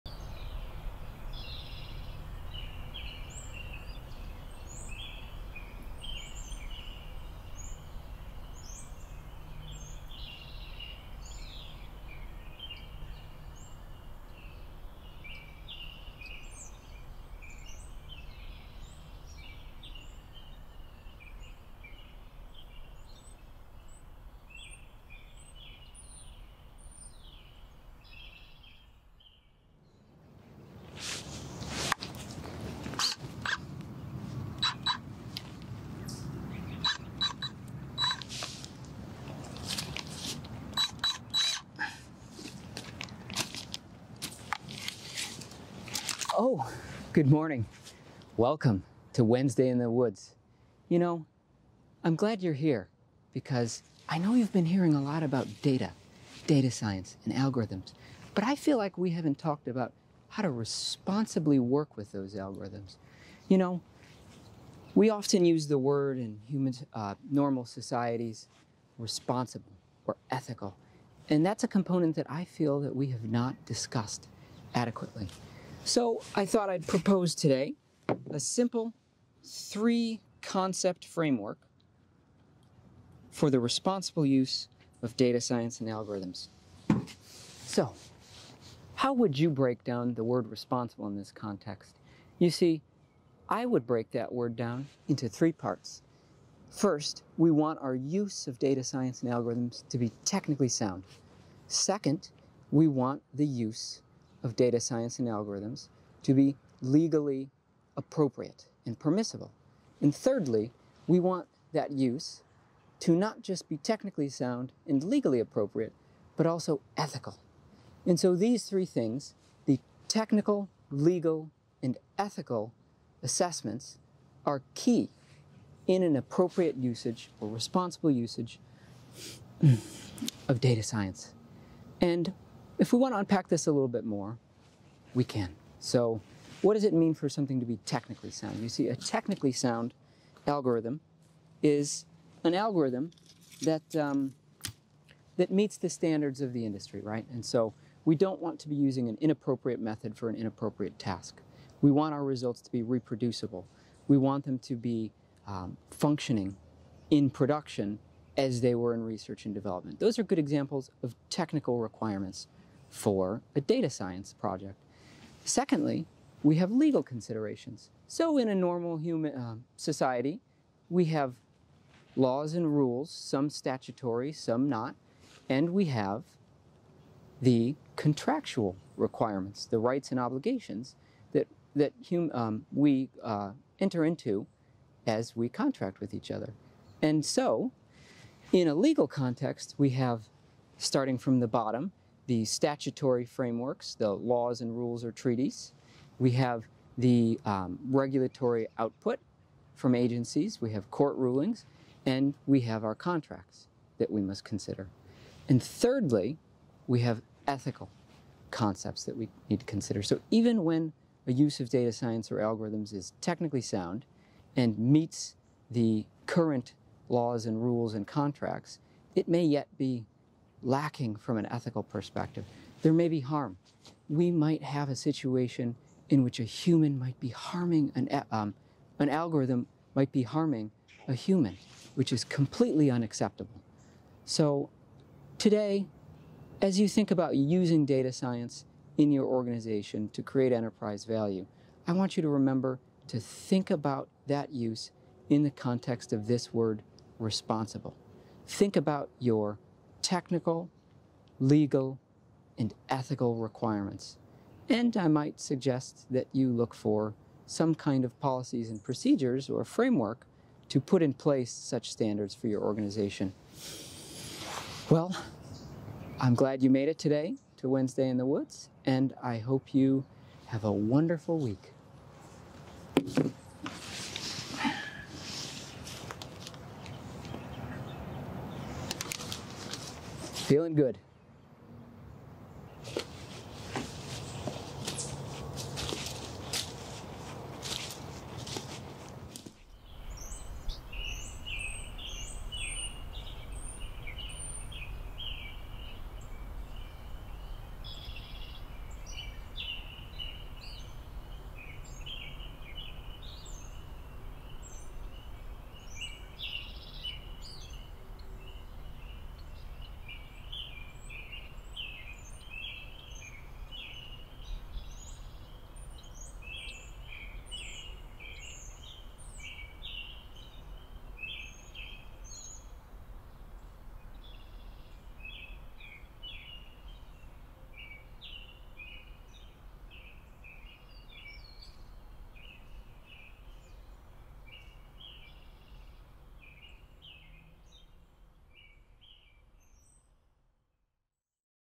Wednesday in the Woods 004: Responsible Data Science
On this week's Wednesday in the Woods, we enjoy a conversation about responsible datascience and algorithms as you join me for my completely normal human morning routine.

If you felt like our discussion of data science and datastrategy previously missed out on pieces of the big picture, you'd be right. That's because we've been focused on some of the traditional management perspectives around "digital" or "AI."

Today, we're taking a moment to unpack the word "responsible" in this context. Specifically, we propose that responsible data science considers three factors:

Technical factors are the ones you'd expect an organization to focus on first. Are we using a clustering algorithm for a regression problem? Are we using the right data store or production implementation architecture? Is our model overfit or underfit?

Legal factors are often next for an organization to consider. Is our use of data within the rights and obligations of our contracts? Are there any statutory frameworks or treaties that obligate us to notify a party or prohibit a behavior? Has a court set a precedent for damages to consider?

Lastly, and most relevant to recent conversation in society, is whether such use of data science and algorithms are "ethical." Data science projects can be technically sound and not in violation of public laws or contracts, yet still violate ethical principles or make people feel off.

Not-so-coincidentally, we recently released a Responsible Data Science Policy framework. If you're interested in more "real" contents on the topic, head on over to our article introducing the framework